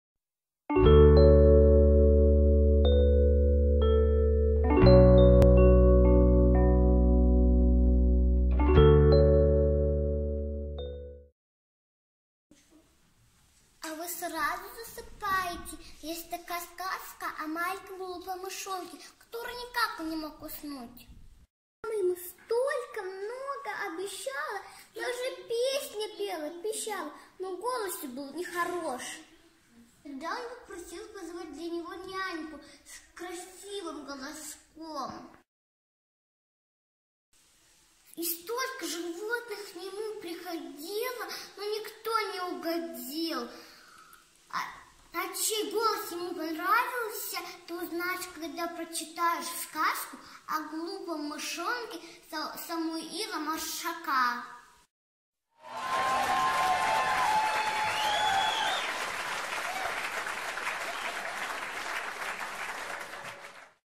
С.Я.Маршак "Сказка о глупом мышонке"
Ефимова Алиса, 7 лет.
ФИО куратора: Кокарева Евгения Александровна, учитель-логопед
Место работы: г. Архангельск МБДОУ Детский сад №186
Комментарий: Буктрейлер  снят  с участием детей подготовительной к школе группы к произведению С. Я. Маршака. Просматривали буктрейлеры дошкольники, которые ещё не читали творчество данного автора. Всем без исключения захотелось познакомиться с произведениями Самуила Маршака.  Дети с удовольствием включились в мини –инсценировку.